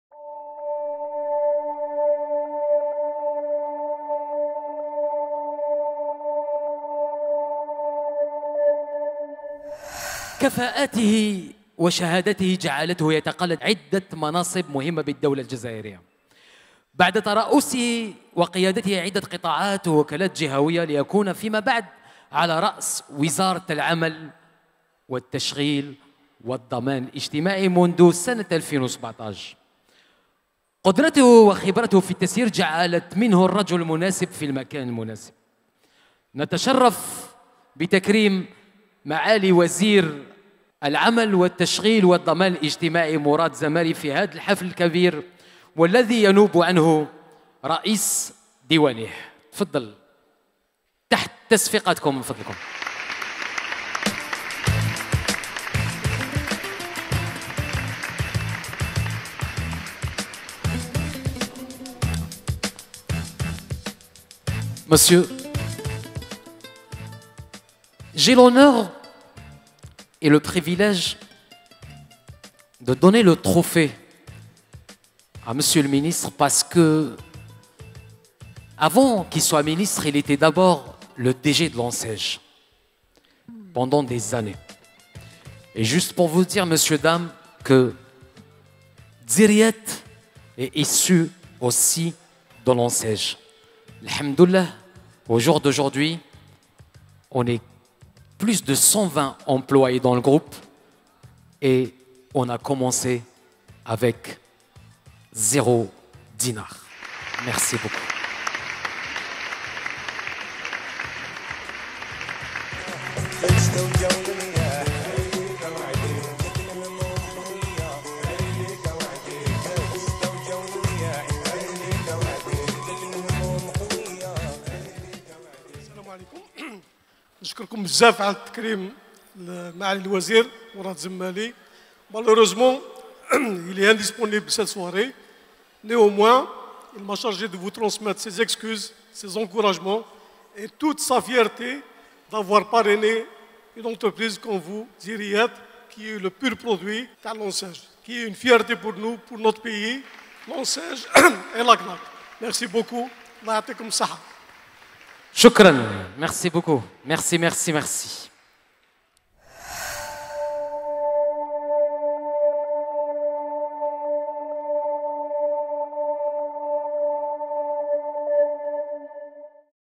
Mourad Zemali, ministre du Travail, de l’Emploi et de la Sécurité sociale sacré dziri d or 2018
Mourad Zemali, ministre du Travail, de l’Emploi et de la Sécurité sociale

Ministre du Travail, de l’Emploi et de la Sécurité sociale, M. Mourad Zemali a été parmi les personnalités récompensées par Dziri Magazine. Avant de devenir ministre en 2017, Mourad Zemali a occupé plusieurs postes dans plusieurs domaines, essentiellement dans le travail. Son trophée a été remis à son représentant, le chef de cabinet du ministère, M. Nasreddine Bouguerra.